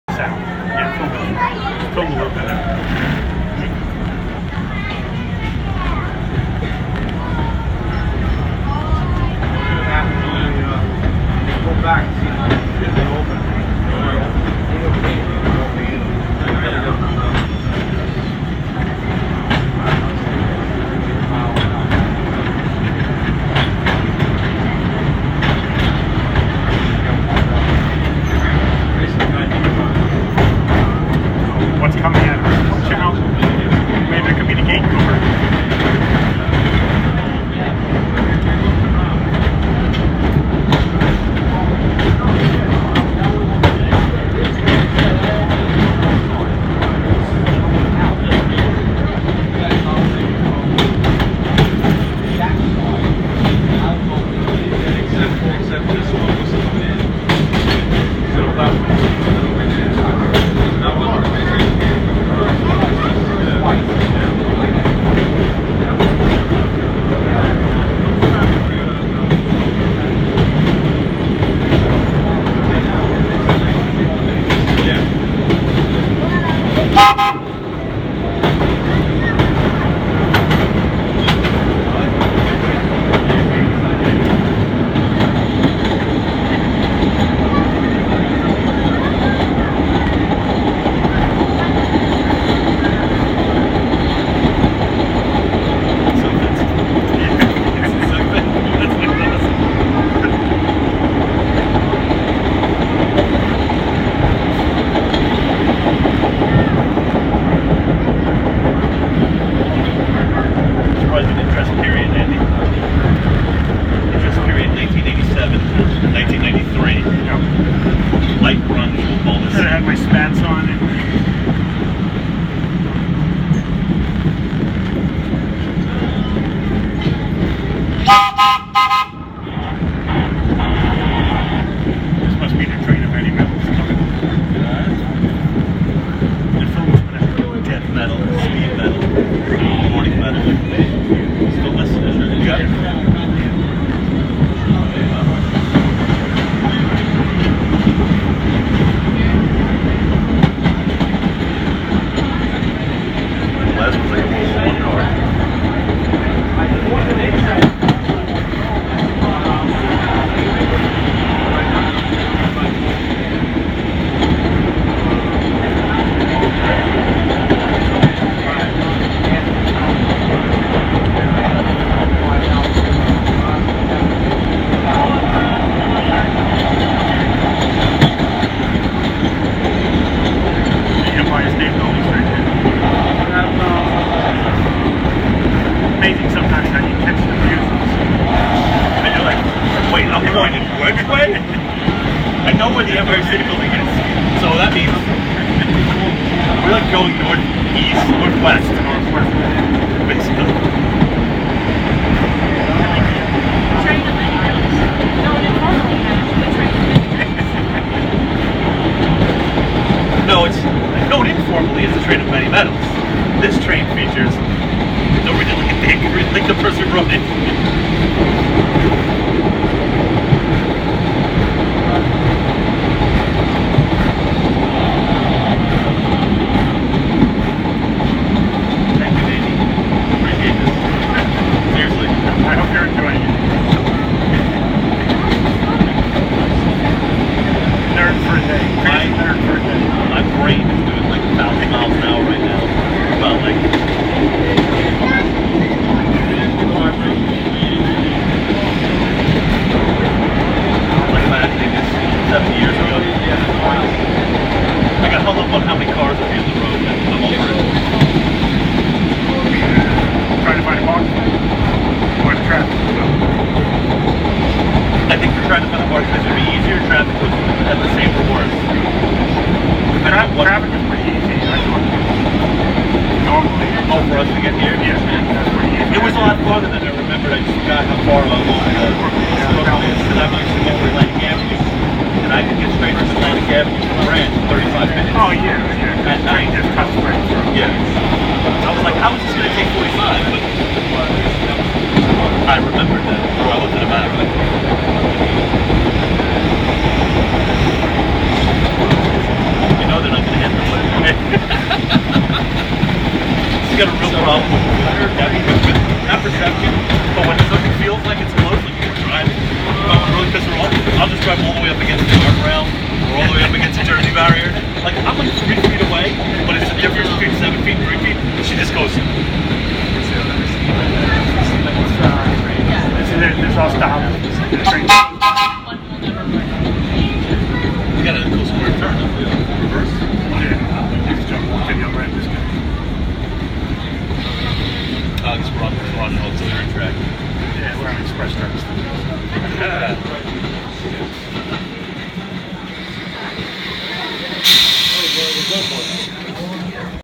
Front Window! BMT Centennial From Brighton Beach to Kings Highway
Every year, the MTA has an event when the trains in the museum are put into service. This year, there were 7 vintage cars.
I got to take a video from the front of a BMT Centennial.

Taken on June 17th 2018